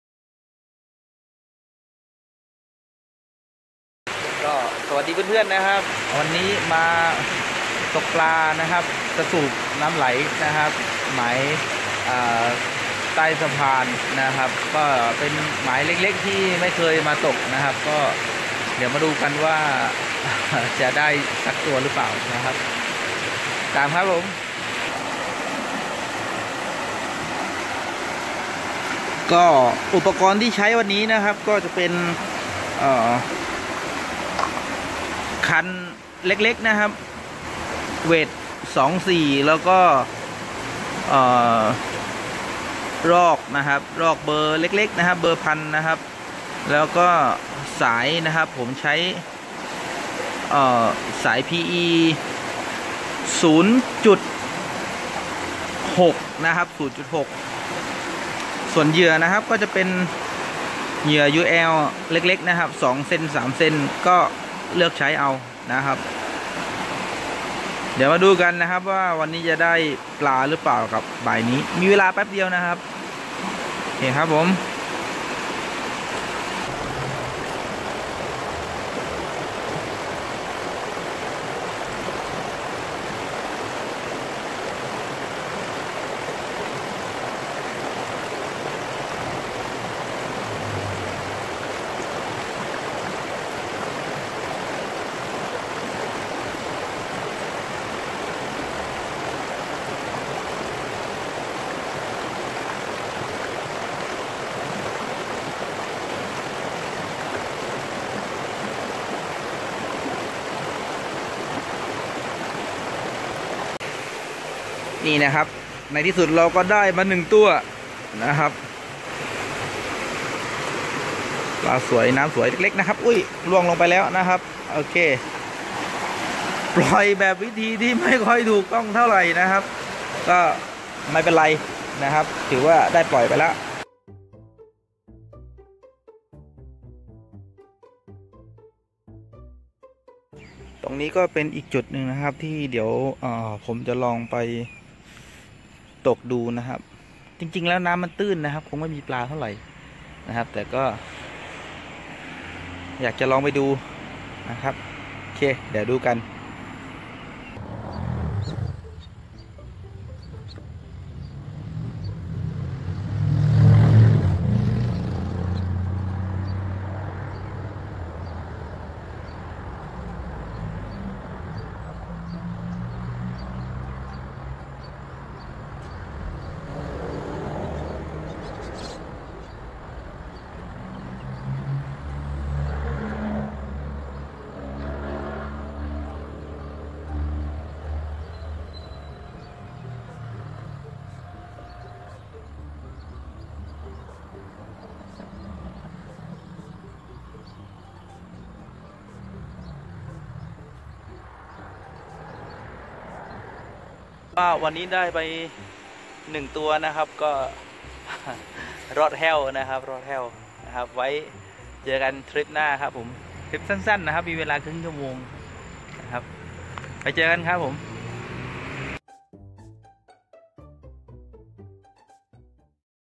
ตกปลาด้วยชุด UL กระสูบน้อยใต้สะพาน กระบี่
ตกปลาด้วยชุด UL กระสูบน้อยใต้สะพาน กระบี่ ชุดอุปกรณ์เล็กๆ สนุกดีไปอีกแบบครับผม